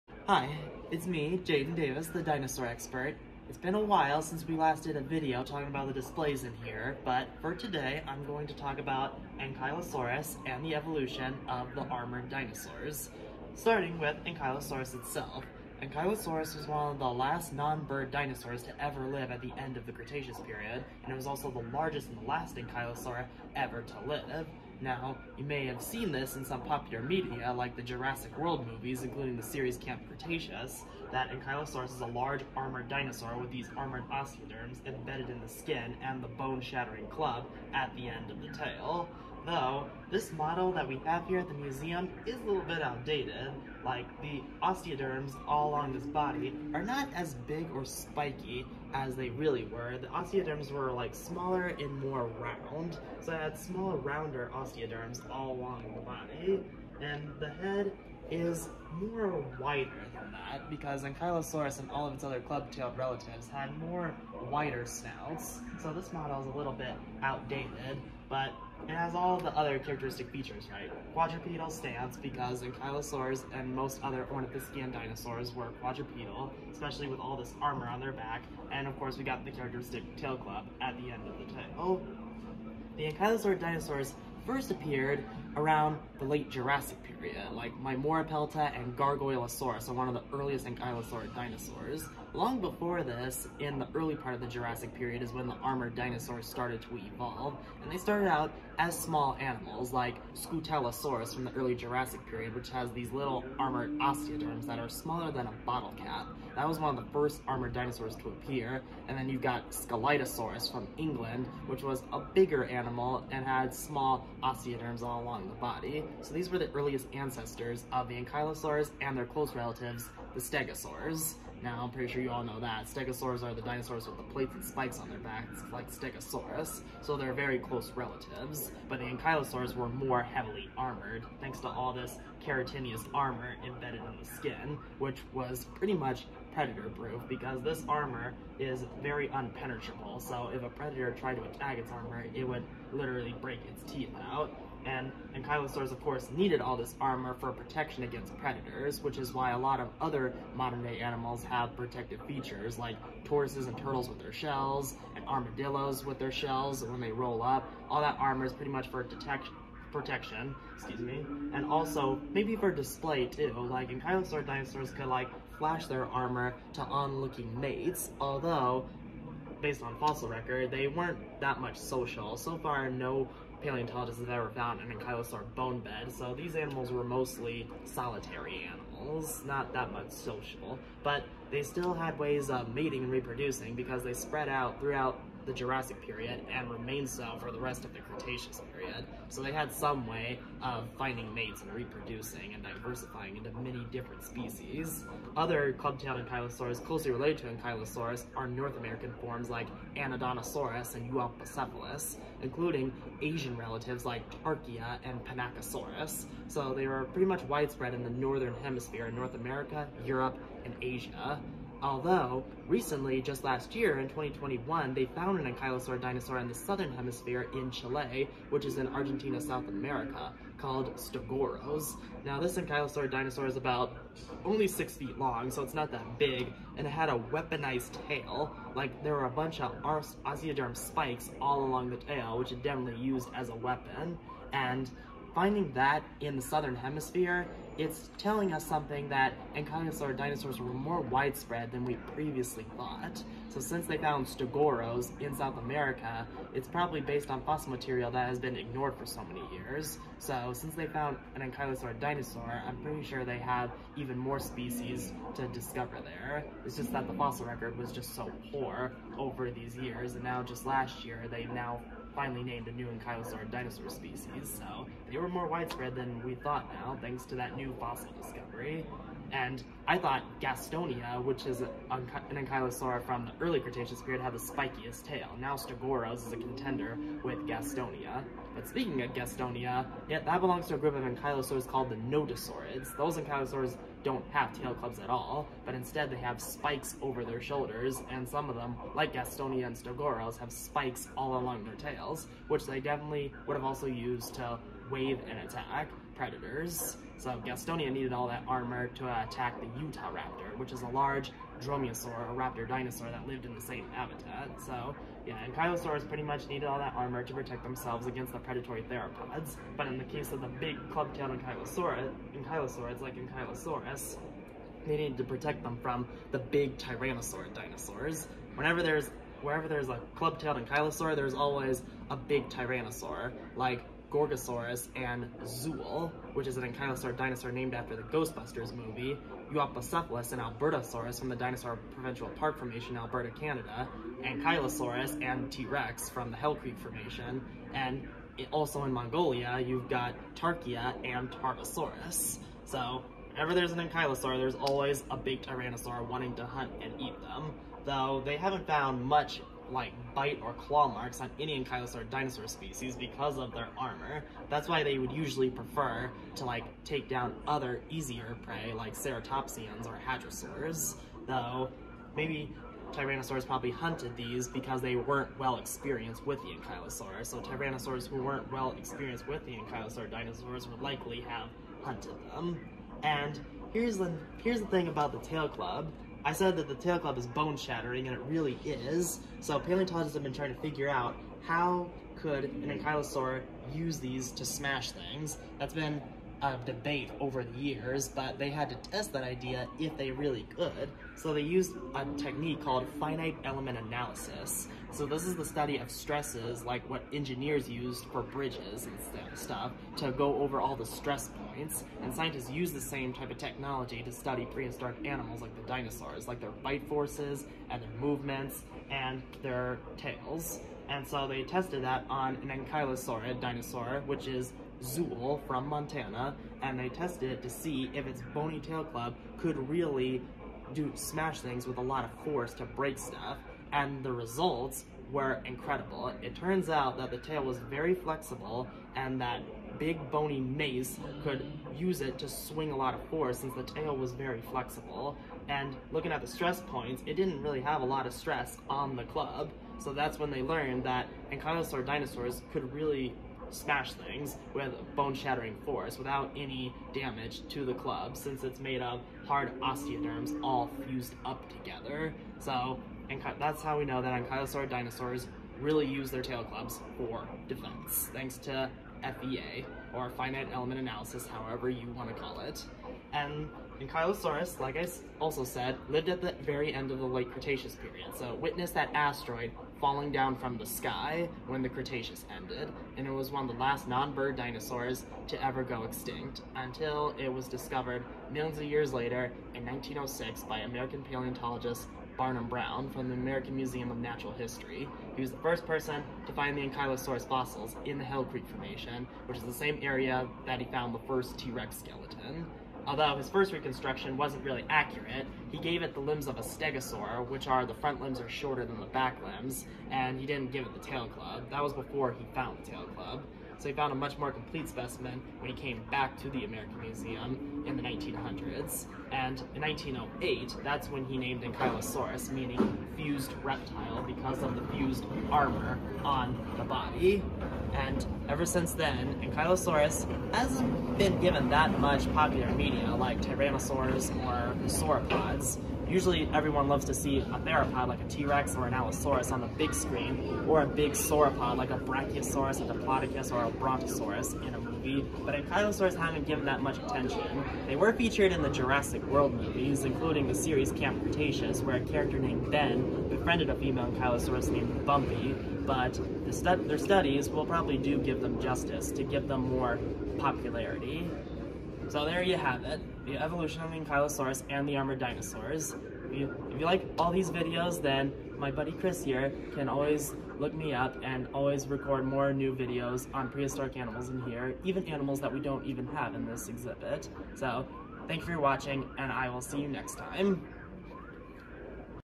Learn about Ankylosaurus
Dino corner with Jadon, our resident “Dino expert”, talks all things Ankylosaurus. Straight from the Las Vegas Natural History Museum’s Prehistoric Gallery, learn along with him as he delves into what makes this dinosaur one of the most famous yet under-represented when it comes to movies.